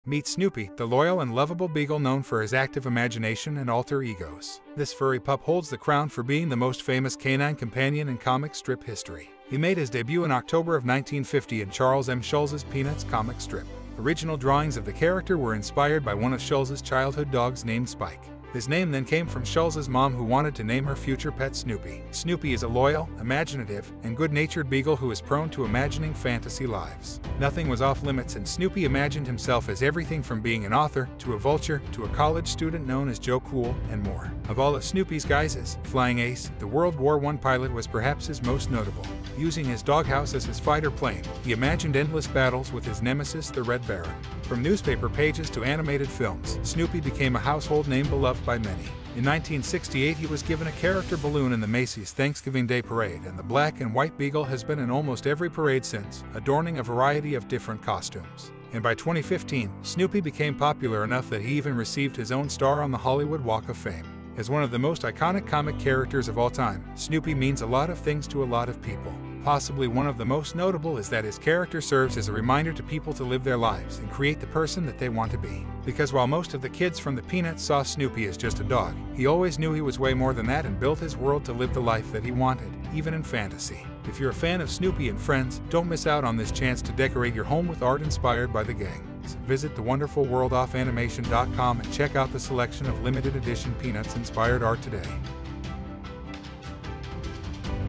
Character Spotlight - Snoopy
The loyal and loveable beagle known for his active imagination and alter egos.